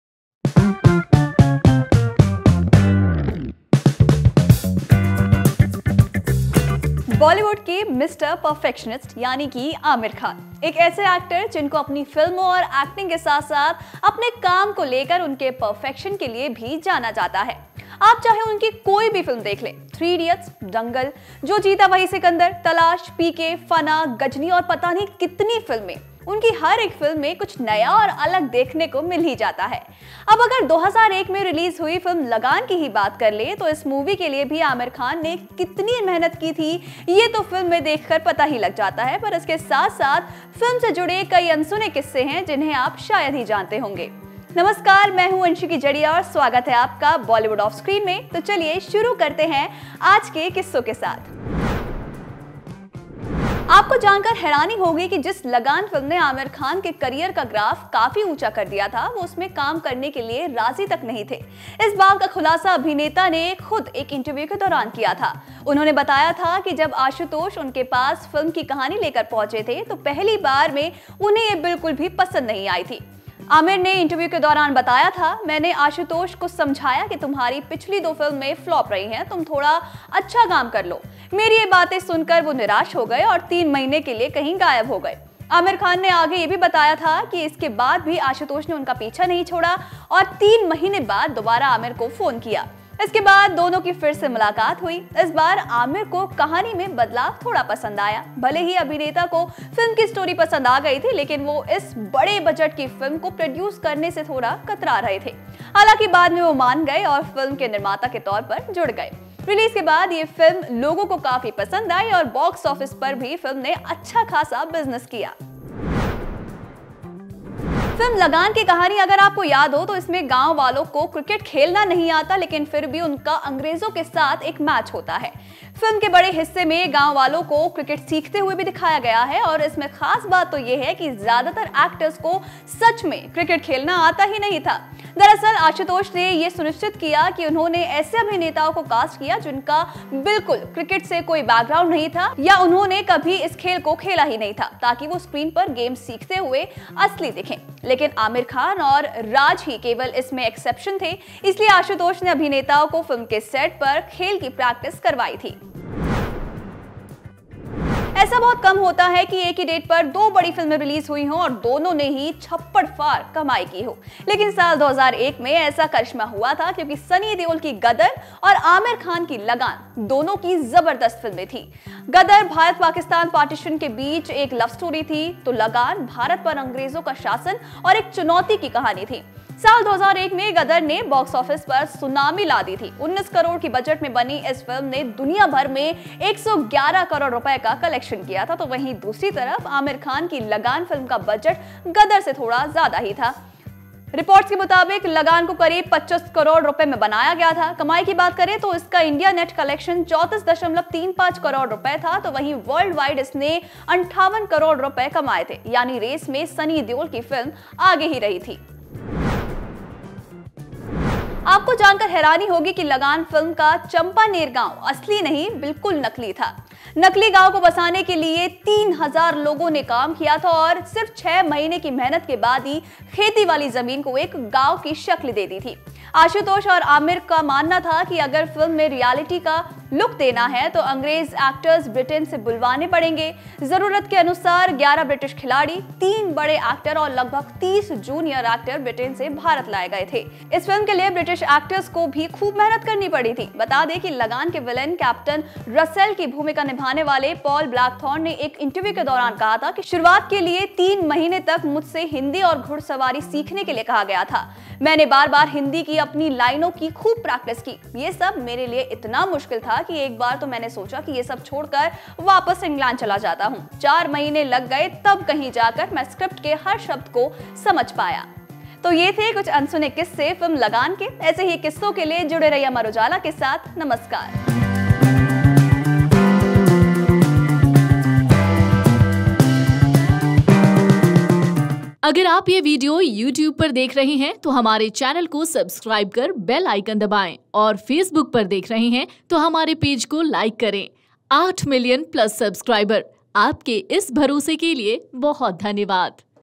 जानें Lagaan से जुड़े वो अनसुने किस्से, जिसे शायद ही आप जानते हो l Aamir Khan l Bollywood Offscreen
अपनी एक्टिंग और फिल्मों के लिए जाने वाले आमिर खान की फिल्म लगान ने बॉक्स ऑफिस पर बहुत अच्छी कमाई की थी, लेकिन क्या हैं इस फिल्म से जुड़े अनसुने किस्से, जानें इस वीडियो में